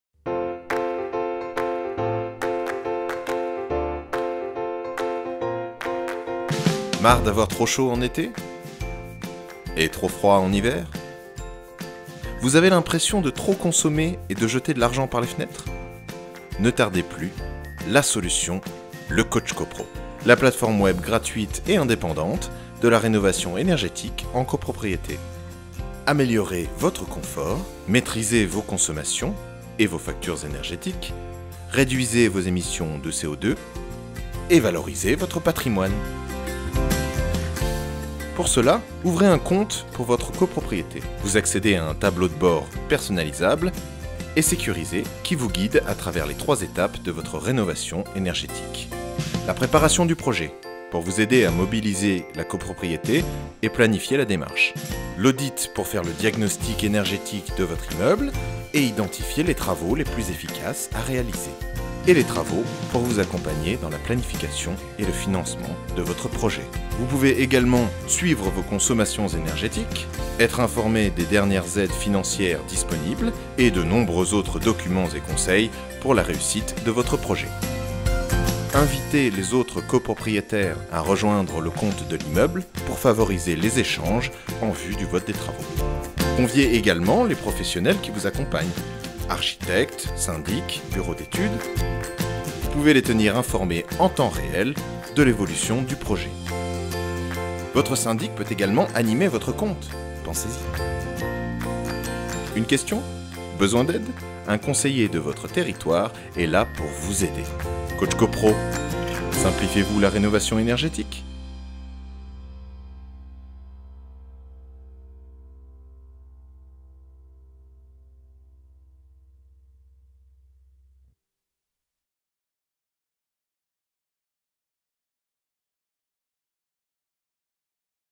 Découvrez le CoachCopro® en quelques secondes
Vous cherchez de l'aide, des conseils objectifs pour engager ou conduire un projet de travaux de  rénovation dans votre copropriété ? Faites vous accompagner ! La solution : le CoachCopro® la plateforme web spécialisée sur la rénovation énergétique en copropriété.Découvrez toutes les fonctionnalités du site  CoachCopro spécialement conçu pour aider les copropriétaires et les syndics à toutes les étapes du projet de rénovation de la copropriété, depuis la réalisation de l'audit , jusqu'aux travaux d'isolation en passant par la phase de recherches de financements des travaux. La plateforme CoachCopro® est un service d'intérêt général développé par l'Agence Parisienne du Climat et mis gratuitement à disposition des copropriétaires et des syndics.